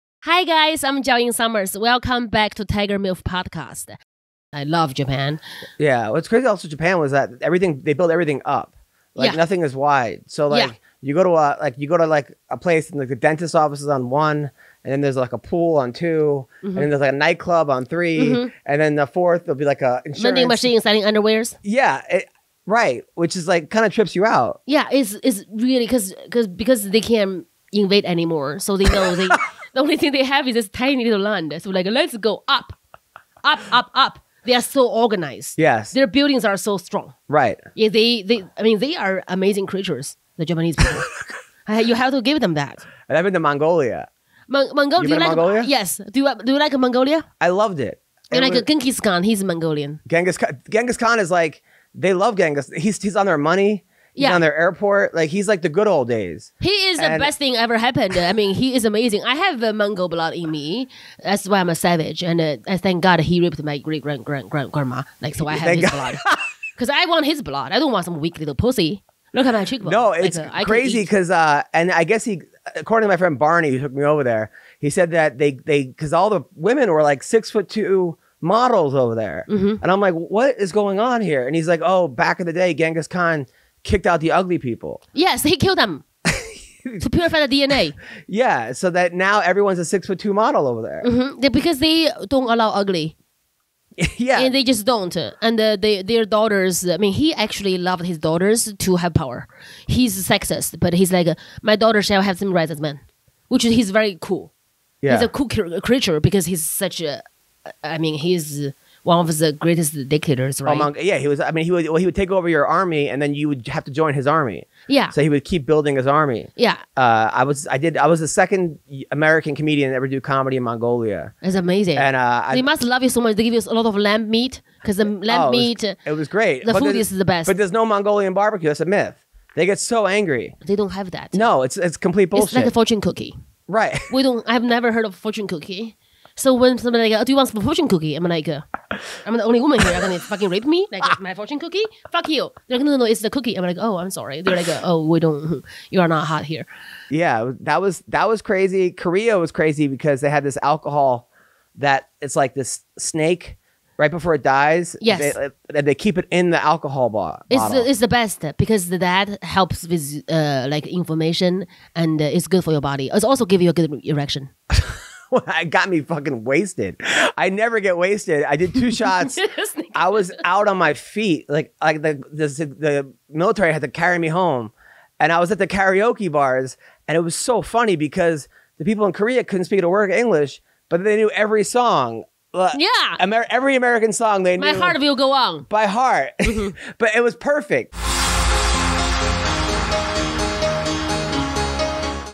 Ghengis Kahn's DNA!! Why Jiaoying Is SOOO Savage!!
Jiaoying and Adam Huter start off talking about Japan and how they build everything up. Jiaoying jokes its because they can't invade anymore so they just build up. Jiaoying has Mongolian blood in her and that is why she is a savage. She says thank god he raped her great great grandmother. Adam says everyone over there is 6 foot 2 model and Jiaoying says its because Genghis Kahn killed all the ugly people.